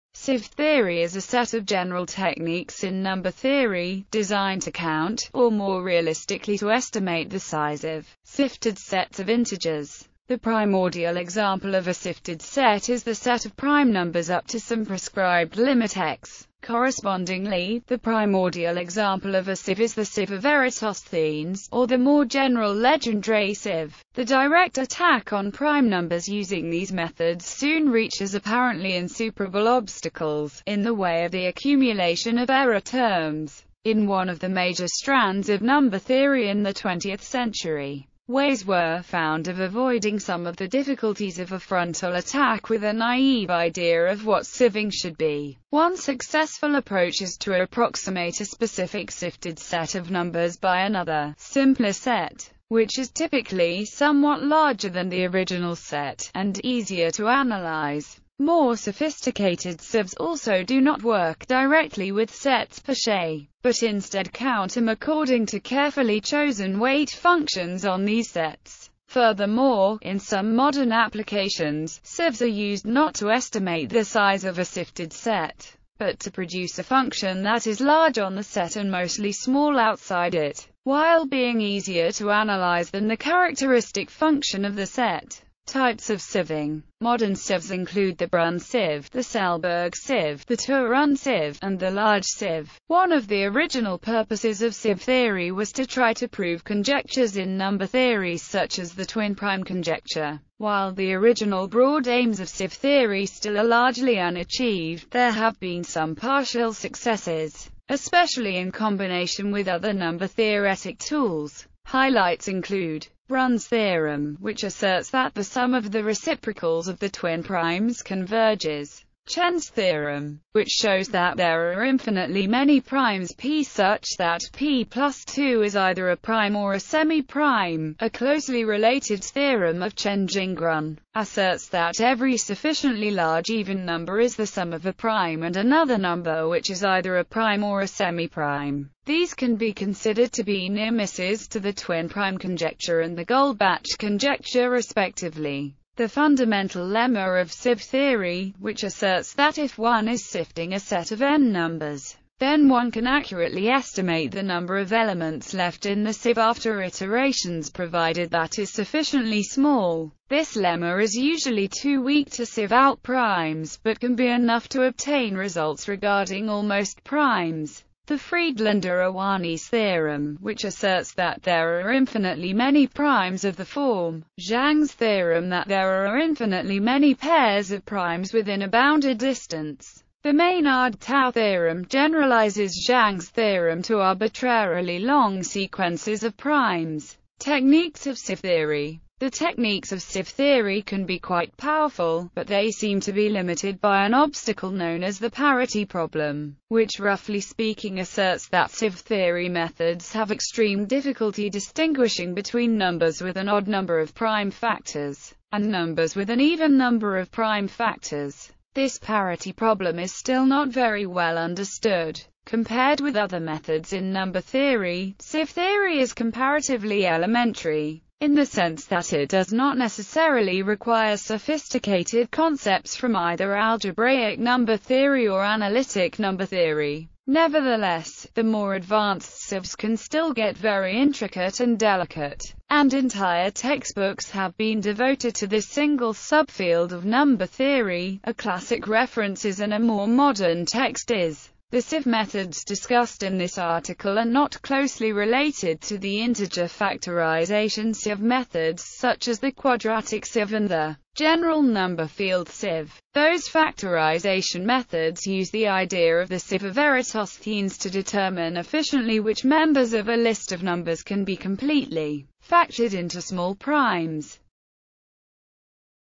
Sieve theory
Sieve theory is a set of general techniques in number theory, designed to count, or more realistically to estimate the size of, sifted sets of integers.The primordial example of a sifted set is the set of prime numbers up to some prescribed limit X.